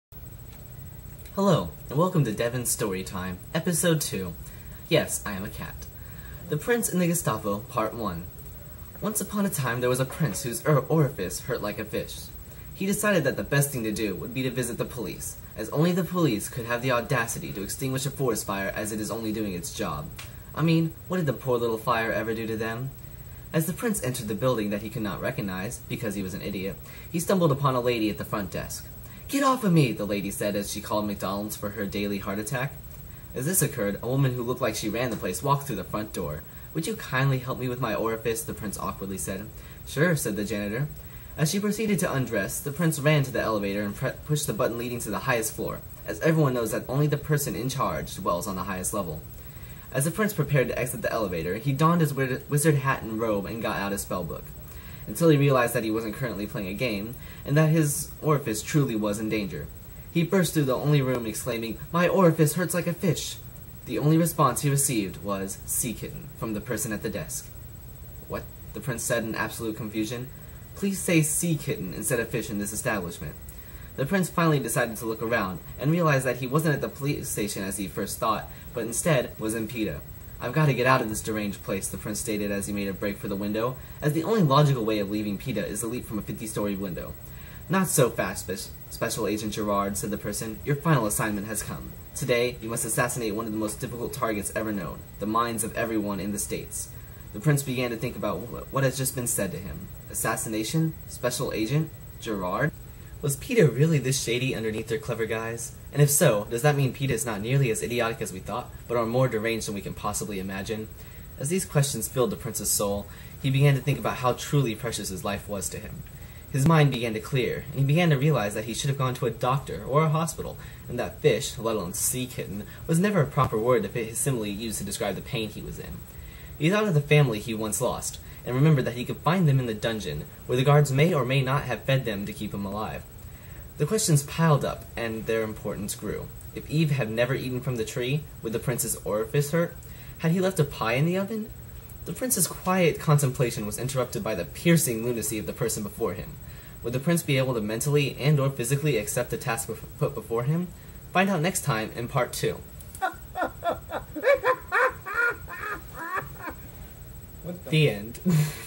Devon's Story Time- The Prince and the Gestapo pt.1
Episode 2 of Devon's phenomenal story time. Enjoy yet another amazing and awe inspiring story...

The Prince and the Gestapo Pt. 1
Once upon a time there was prince whose orifice hurt like a fish.    He decided that the best thing to do would be to visit the police, as only the police could have the audacity to extinguish a forest fire as it is only doing its job.  I mean what did the poor little fire ever do to them?
As the prince entered the building that he could not recognize, because he was an idiot, he stumbled upon a lady at the front desk.
Get off of me! the lady said as she called McDonalds for her daily heart attack.  As this occurred, a woman who looked like she ran the place walked through the front door. Would you kindly help me with my orifice? the prince awkwardly said.  Sure said the janitor.  As she proceeded to undress, the prince ran to the elevator and pushed the button leading to the highest floor, as everyone knows that only the person in charge dwells on the highest level.
As the prince prepared to exit the elevator, he donned his wizard hat and robe and got out his spell book.  Until he realized that he wasnt currently playing a game, and that his orifice truly was in danger.  He burst through the only room exclaiming My orifice hurts like a fish!  The only response he received was Sea Kitten, from the person at the desk.  What? the prince said in absolute confusion.  Please say sea kitten instead of fish in this establishment.  The prince finally decided to look around, and realized that he wasnt at the police station as he had first realized, but instead, was in PETA.
Ive got to get out of this deranged place, the prince stated as he made a break for the window, as the only logical way of leaving PETA is to leap from a 50 story window.  Not so fast Special Agent Gerard, said the person, your final assignment has come.  Today, you must assassinate one of the most difficult targets ever known: the minds of everyone in The States.  The prince began to think about what has just been said to him.  Assassination, Special Agent, Gerard?
Was PETA really this shady underneath their clever guise?  And if so, does that mean PETA is not nearly as idiotic as we thought, but are more deranged than we can imagine?  As these questions filled the princes soul, he began to think about how truly precious his life was to him.  His mind began to clear, and he began to realize that he should have gone to a doctor or a hospital, and that fish, let alone sea kitten, was never a proper word to fit his simile used to describe the pain he was in.  He thought of the family he once lost, and remember that he could find him in the dungeon, where the guards may or may not have fed them to keep them alive.  The questions piled up, and their importance grew.  If Eve had not eaten from the tree, would the princes orifice hurt?  Had he left a pie in the oven?
The princes quiet contemplation was interrupted by the piercing lunacy of the person before him.  Would the prince be able to mentally and/or physically accept the task put before him?  Find out next time, in part two.